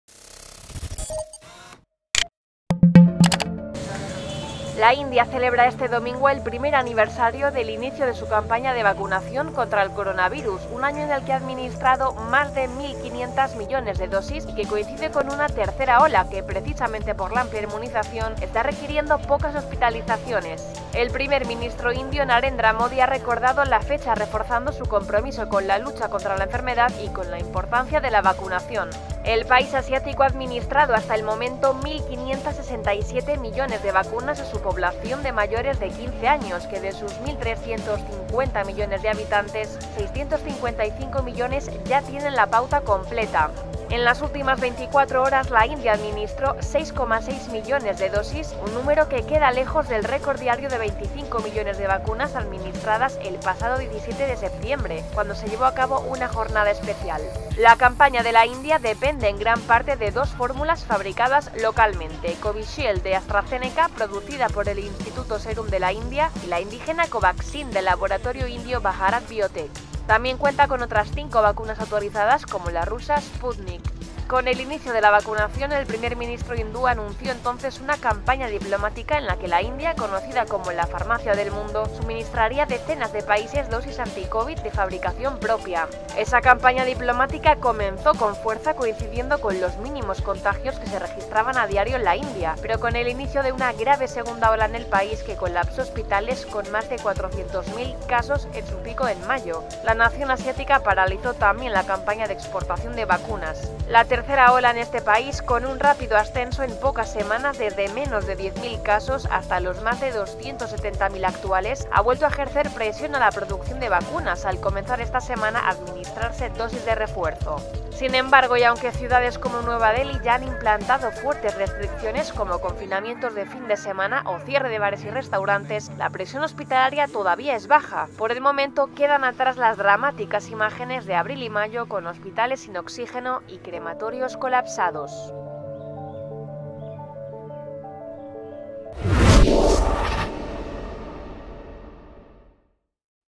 India celebra un año de su campaña de vacunación
India celebró este domingo el primer aniversario del inicio de su campaña de vacunación contra el coronavirus, un año en el que ha administrado más de 1,500 millones de dosis.